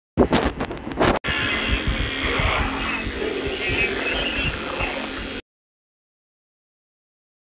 Folarin show very soon atrociraptor
you guy sorry be long after make my trailer so new leaked scene atrociraptor was open  in a cage watch toady and tomorrow tarlier coming april 15 9 episodes my series.